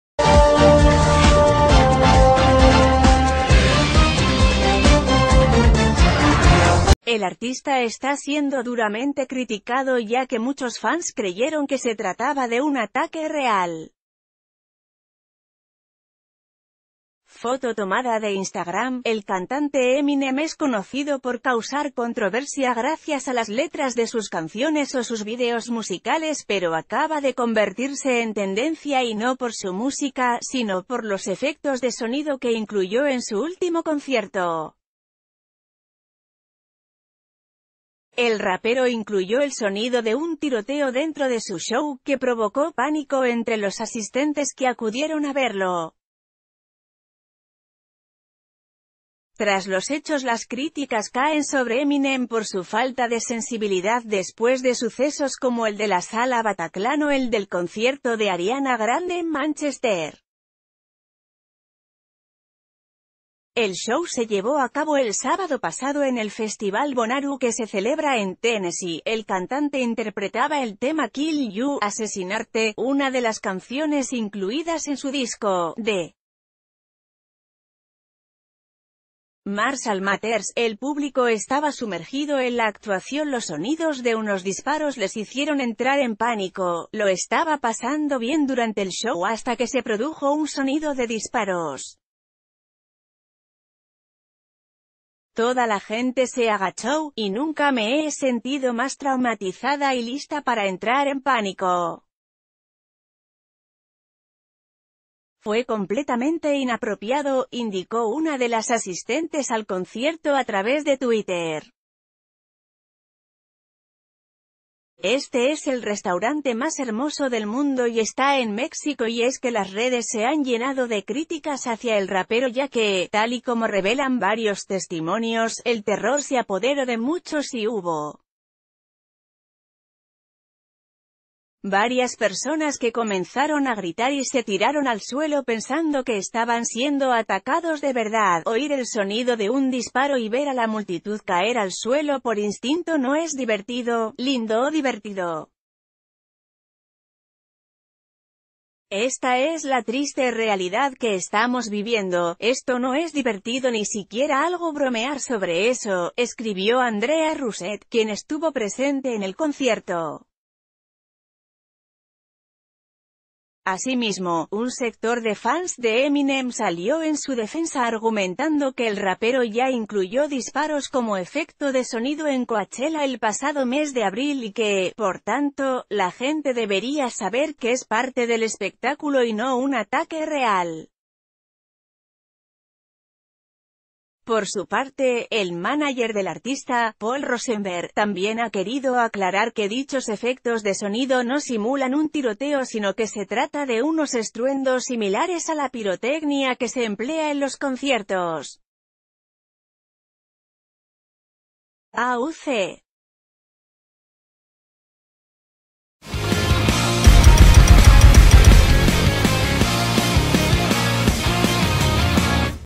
Eminem provoca pánico a fans por irresponsable efecto de sonido
El artista está siendo duramente criticado ya que muchos fans creyeron que se trataba de un ataque real. (FOTO TOMADA DE INSTAGRAM)El cantante Eminem es conocido por causar controversia gracias a las letras de sus canciones o sus videos musicales, pero acaba de convertirse en tendencia y no por su m...